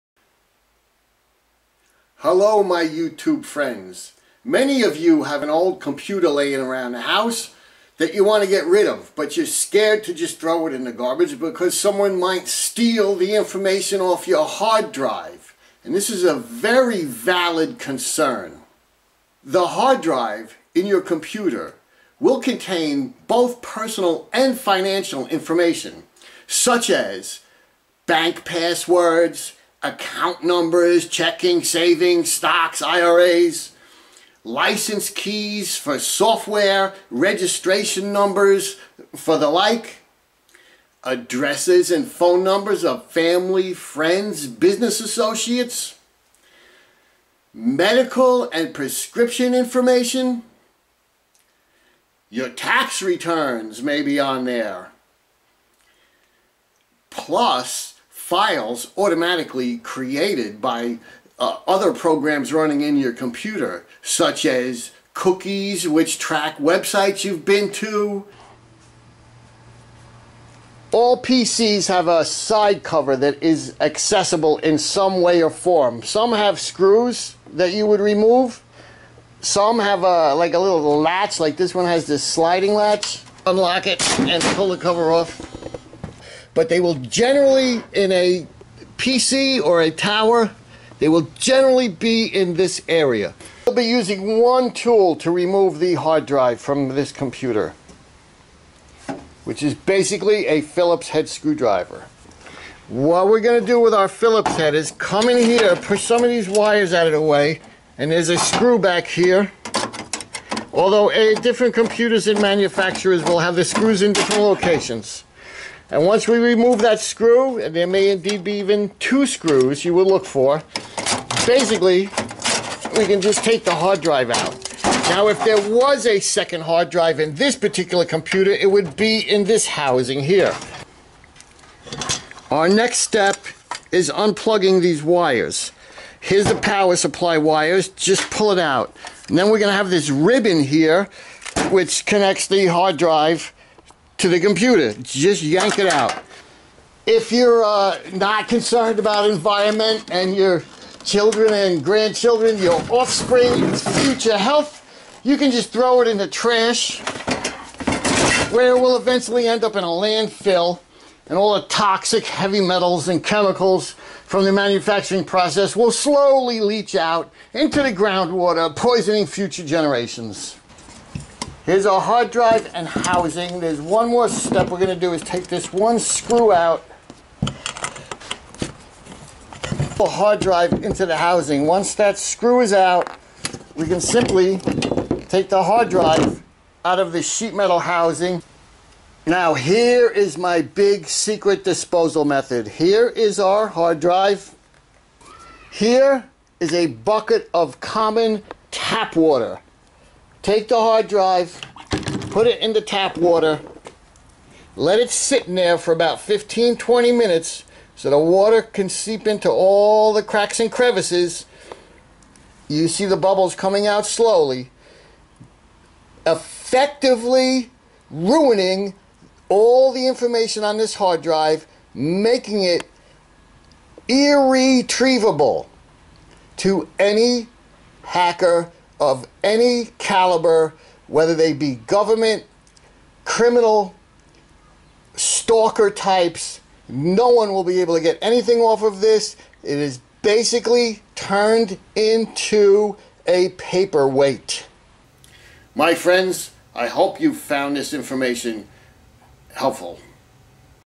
Secure Computer Disposal
PROTECT YOUR  PERSONAL INFO FROM THEFT

Your electronics are a target for thieves looking to steal personal information. What do you do with your old PCs, flash drives or other electronic media when they are outdated and no longer needed? YouTube Channel